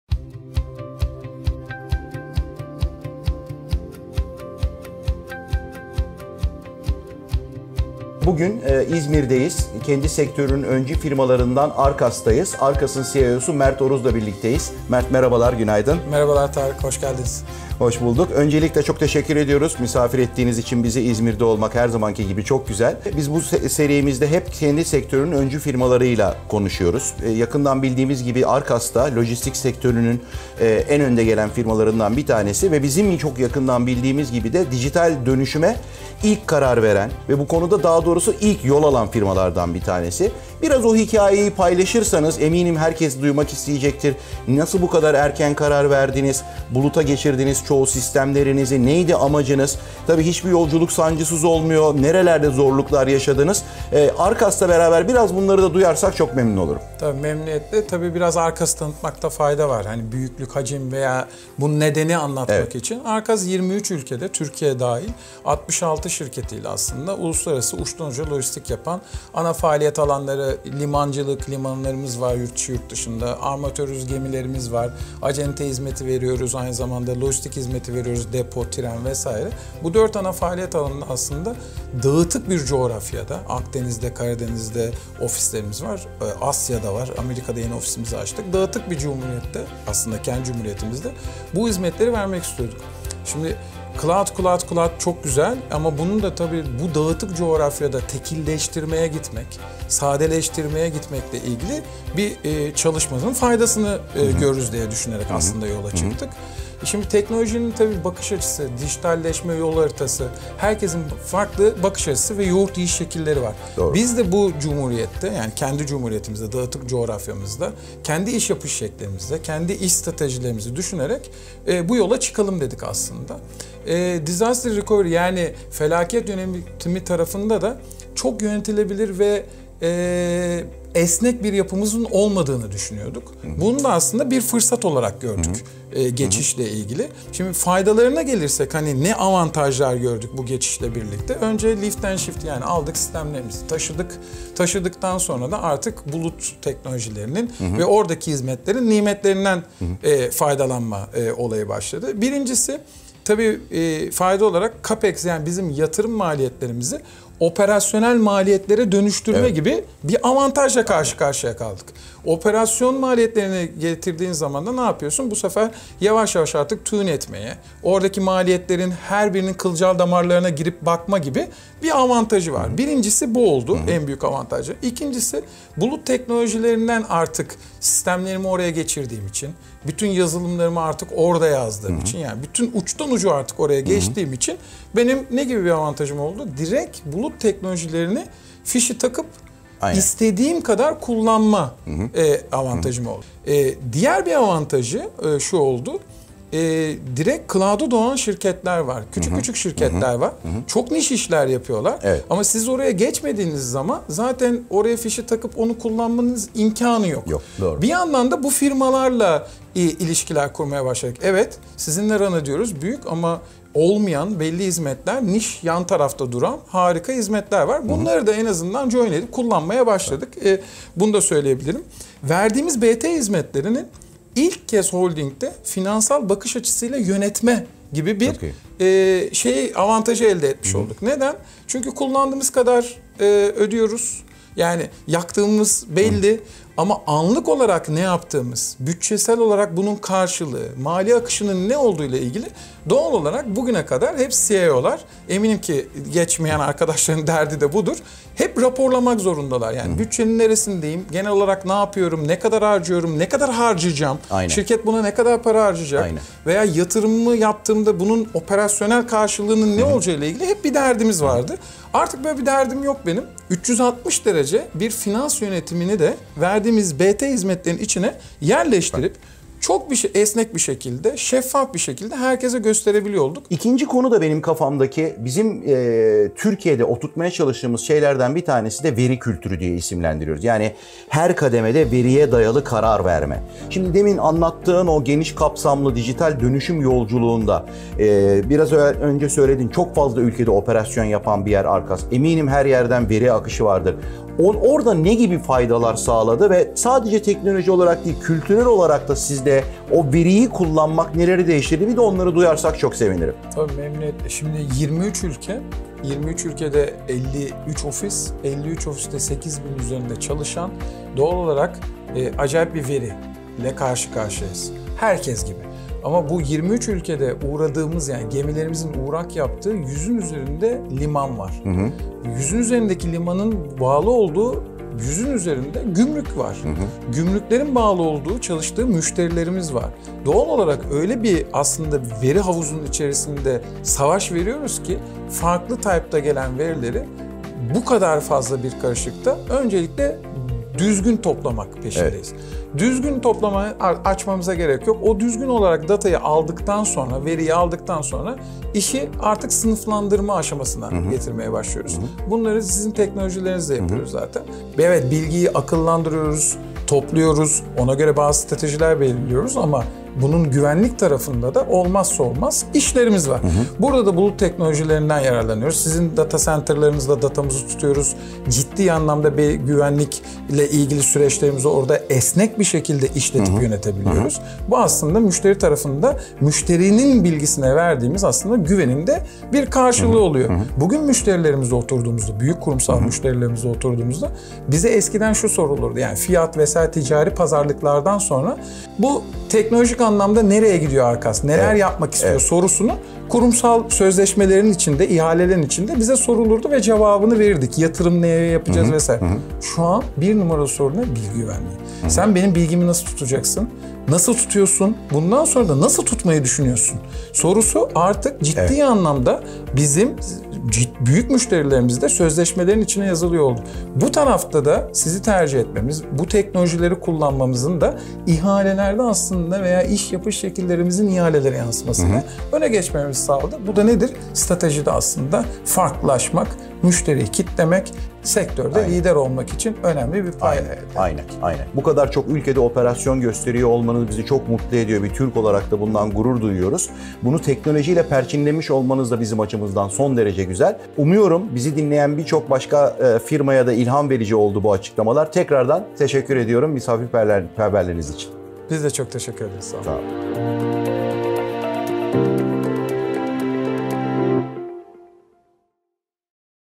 Arkas Holding | Teknoloji ile Birlikte
Türkiye'de kendi sektöründe öncü olmuş ve teknolojiyi uçtan uca ekosistemine entegre ederek en iyi uygulama örneklerini sergileyen firmaların hikayelerini Microsoft Türkiye Kurumsal & Kamu Satış GMY Tarık Tüzünsu ile yakın mercek altına alıyoruz.

Türkiye'de veri kültürü kavramını içselleştirmiş, denizcilik ve lojistik sektörü öncü şirketi Arkas Holding'in ilham veren hikayesinin tamamını şimdi izleyin